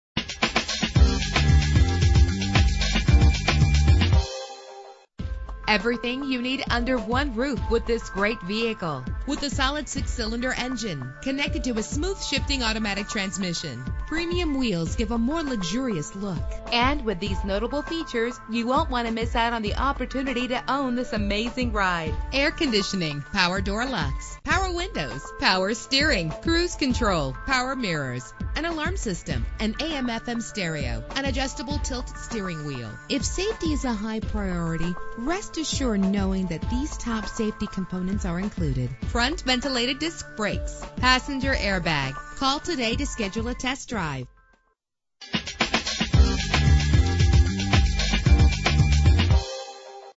2000 Mercury Sable - Gurnee IL
Year: 2000
Make: Mercury
Model: Sable
Trim: LS Premium
Engine: 3.0 liter 6 cylinder 24 valve
Transmission: 4-Speed Automatic
Color: Black
Mileage: 35886
Address:
18834 W Grand Ave